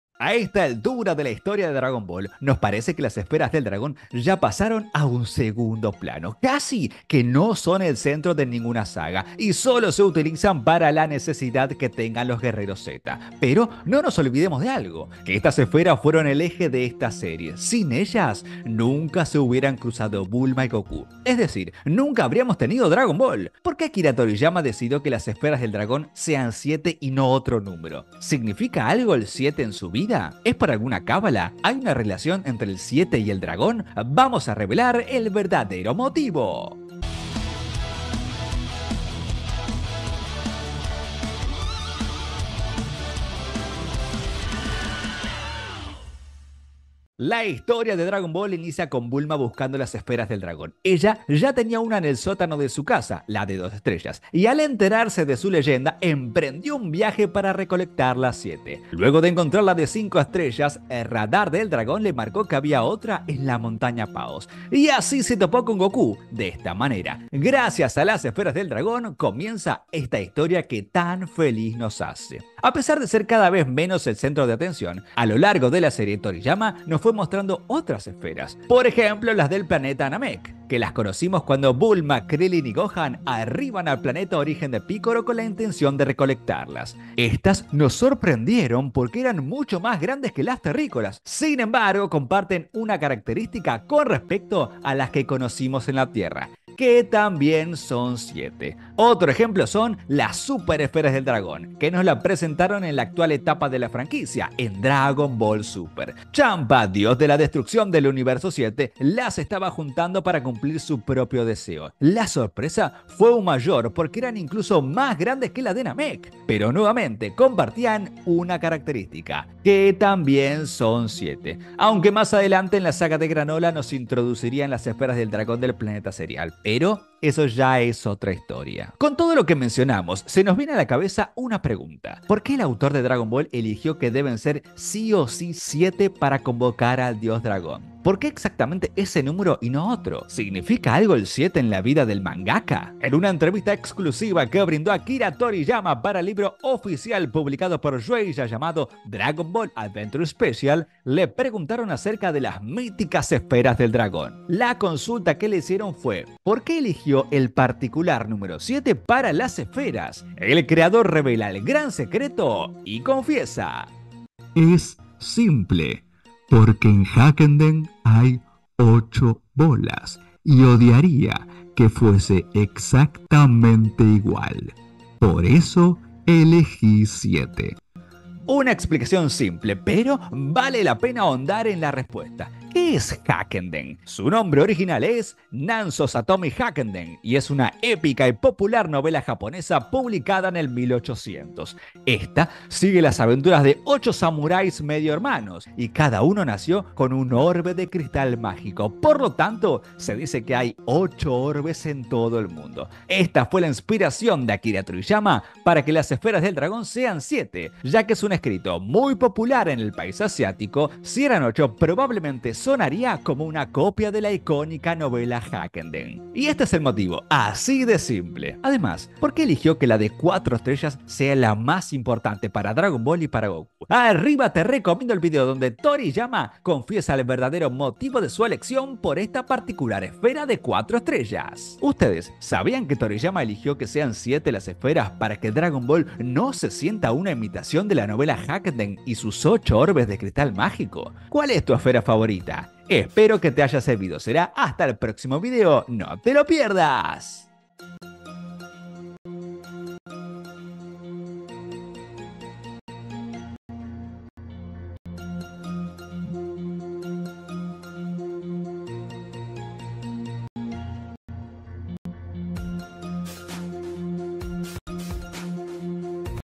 ⍟TORIYAMA confiesa PORQUE son 7 las ESFERAS DEL DRAGÓN en DRAGON BALL | ¿QUÉ es el 7 en SU VIDA?⍟
⍟ENCICLOPEDIA DRAGON BALL: SIEMPRE INFORMACIÓN OFICIAL⍟

Desde los inicios de esta historia, Akira Toriyama dejó en claro que para convocar a Shenlong se deben reunir las 7 Esferas del Dragón. ¿Por qué eligió que sean siete estas bolas mágicas? ¿Significa algo el 7 en Japón? ¿Es un número importante en la vida de Toriyama? En este video, el sensei revela el verdadero motivo de esta elección.